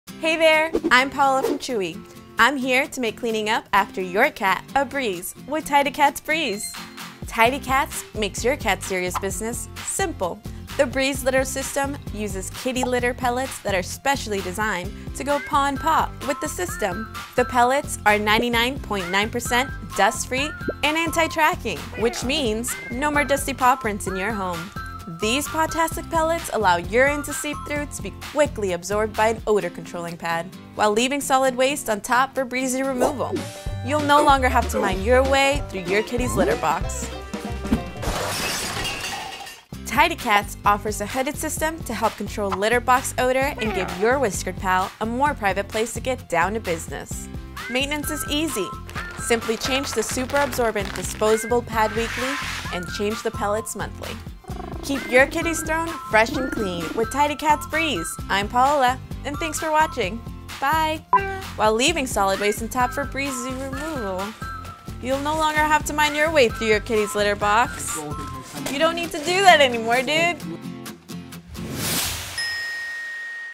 Tidy Cats Breeze | Chewy
Cleaning the cat litter box should be a breeze. Let Tidy Cats Breeze make cleaning your kitty’s litter more pleasant. Its Breeze Cat Litter System uses breeze litter pellets that are specially designed to go paw-in-paw with the Purina Litter Tidy Cats Breeze system. Purina Tidy Cats unique formula contain super absorbent pellets that make it a 99.9 percent dust-free litter and anti-tracking, so no more dusty prints on your house. And if you’re worried about your cat’s business becoming everybody's business, you’ll love how these paw-tastic dust-free cat litter pellets quickly absorb urine to control odor and leaves solid waste on top right where you can see it. Once your feline friend is done doing the duty, just grab your cat litter scooper and remove the stinky stuff. The secret of this Purina Litter are the Tidy Cat Breeze Pads that uses odor-controlling technology to leave your home smelling fresh and clean. Plus, Tidy Cats Breeze Pads work great for households with multiple feline companions and removing each pad is an effortless process. So say ‘goodbye’ to long hours of mining through your cat’s litter box, and say ‘hello’ to more cuddling time and hassle-free cleaning up after your cat family.
For a fresher and cleaner home, get your paws on some Tidy Cats Breeze!

Visit Chewy.com to browse more of your favorite cat supplies.